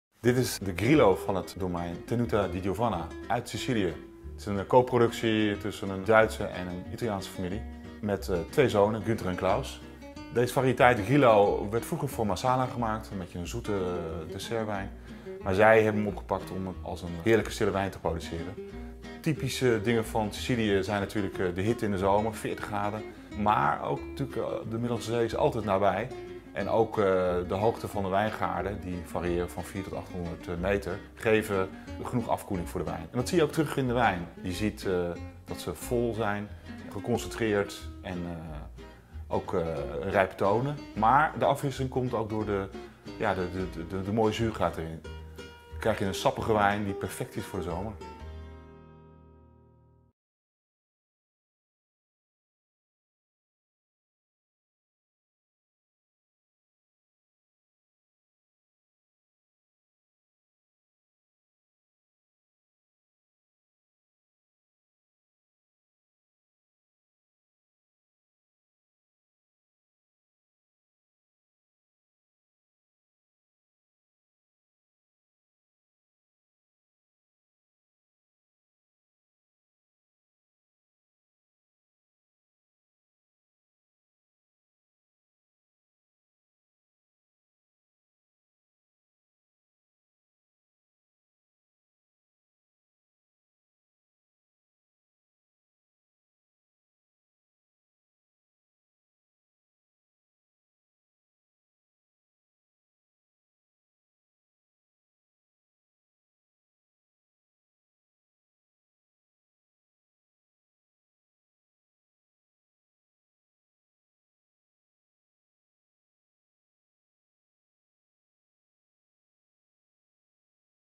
2013 Grillo, Tenuta di Giovanna, Sicily, Italy
Strogeel met licht groene hinten. Een wijn met persoonlijkheid en elegantie. Een van de beste voorbeelden van deze variëteit. Mooie expressie in de neus met rijpe fruitaroma's van perzik en appel, aangevuld met florale hints. De smaak is rond en mondvullend. Perfecte balans tussen sappigheid en zuurgraad. Minerale tonen op de achtergrond. Topprodukt van Günther en Klaus.